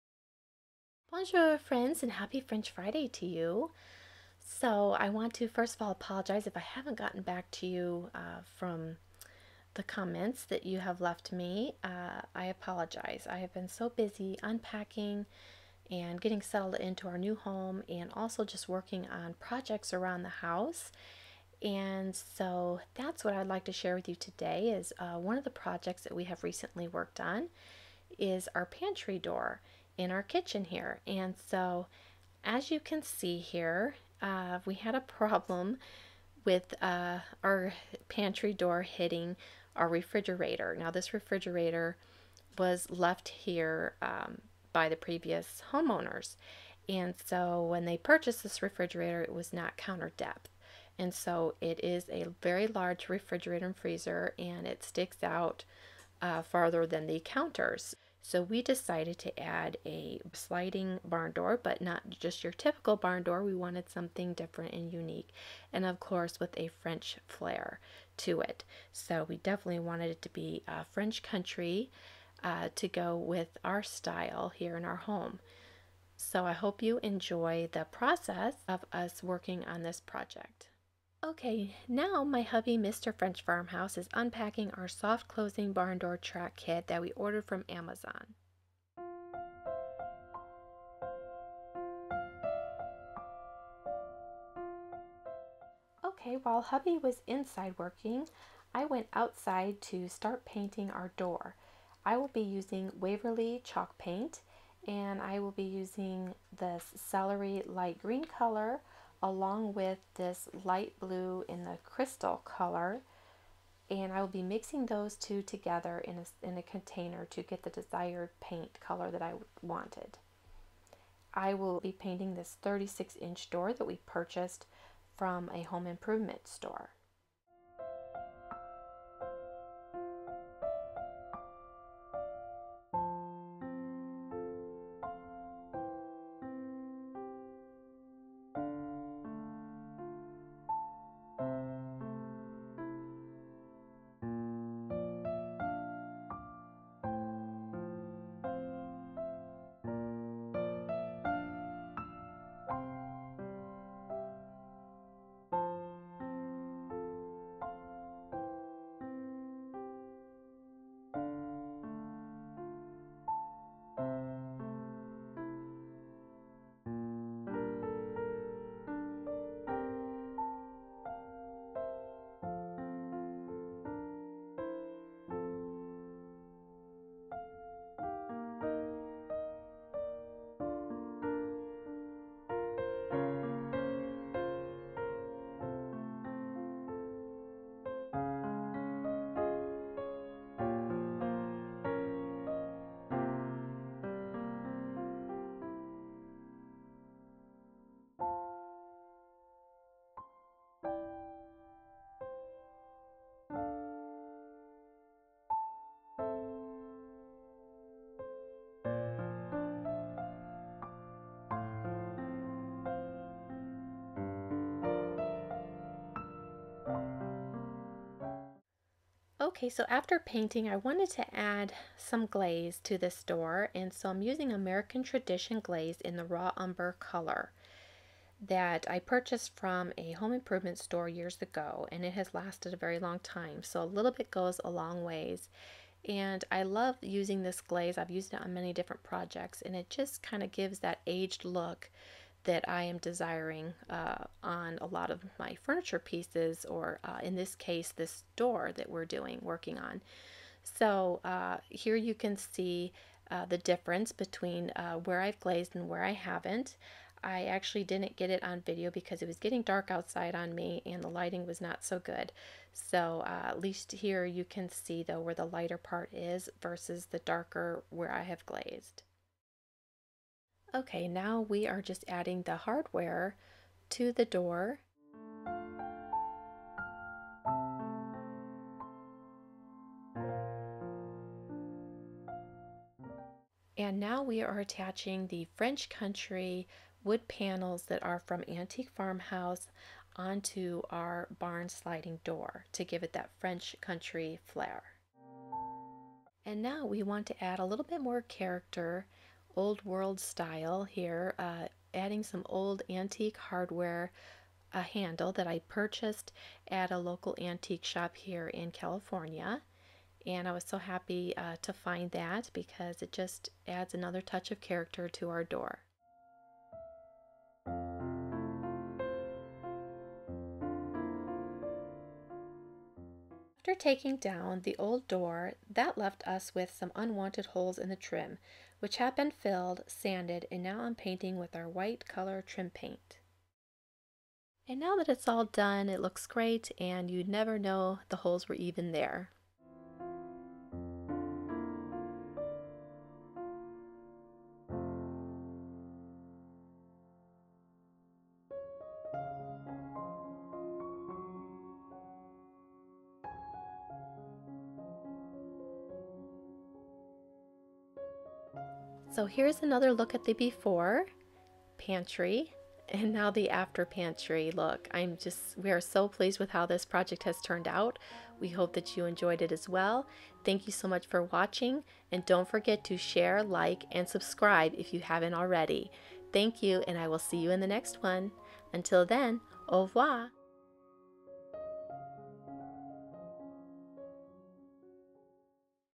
Pantry door makeover ~ French Country Pantry Barn Door
Bonjour friends,

Hi my sweet YouTube friends! I’m so sorry that I’ve not been good lately at responding to comments!! I’ve just been so busy unpacking, organizing, and working on projects around our new home. I hope that you enjoy this French country pantry makeover! We are so happy with the results!!! It’s so much easier to reach things inside our pantry now too.

Love Jessica😘xo

French Country wooden panels used on pantry door are from: Antique Farmhouse